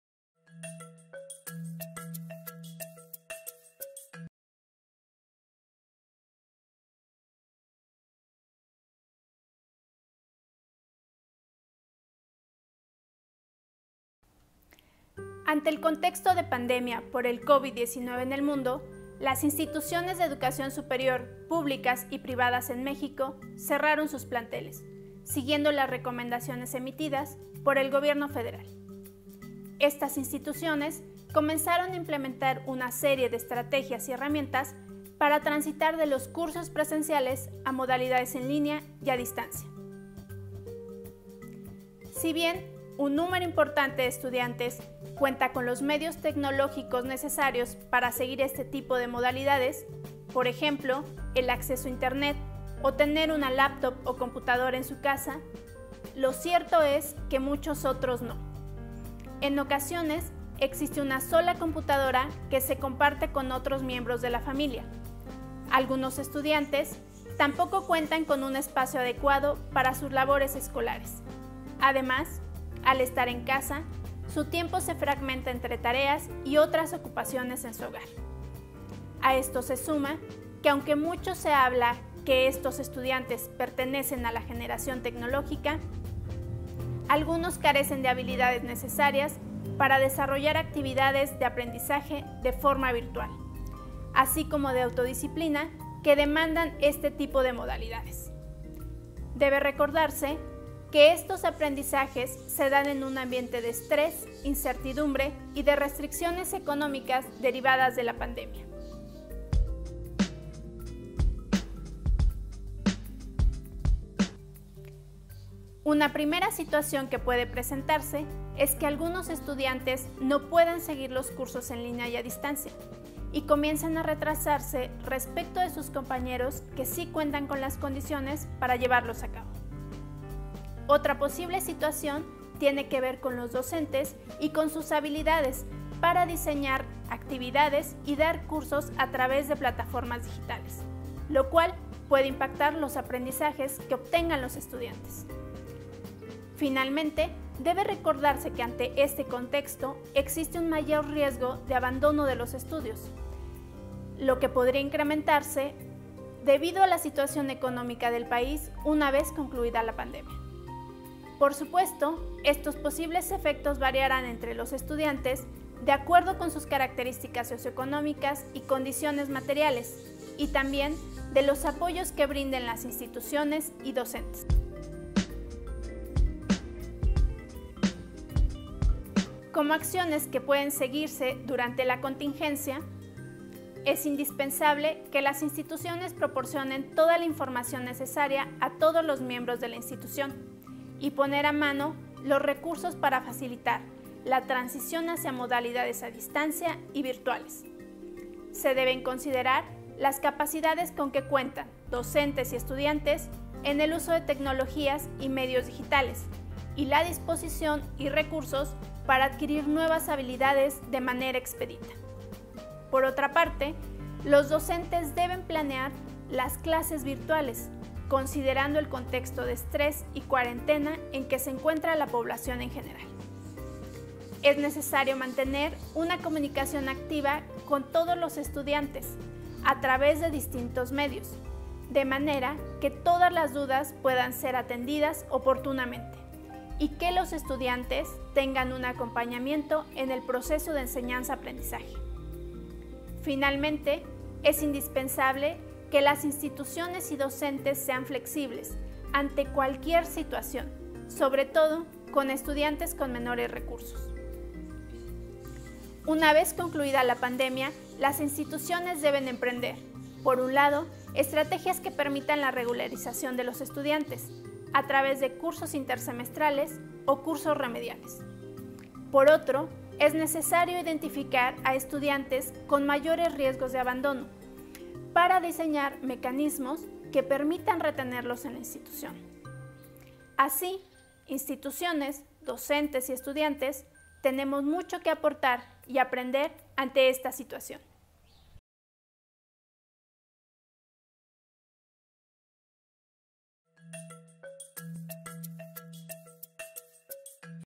Trayectorias escolares ante la pandemia ¿continuar, interrumpir o desistir?
Serie de entrevistas

Educación y pandemia: Tensiones, retos y experiencias

Investigadoras e investigadores del Instituto se han propuesto realizar entrevistas a especialistas en áreas vinculadas a la pandemia, desde una mirada educativa e histórica. En esta serie podrás conversar mediante tus comentarios, con quienes realizan investigación desde el IISUE.

En el video titulado “Trayectorias escolares ante la pandemia ¿continuar, interrumpir o desistir?”  la Dra. Mónica López Ramírez, investigadora del IISUE, hace un balance de las condiciones actuales del estudiantado, docentes y de las responsabilidades de las IES en el actual contexto de emergencia sanitaria.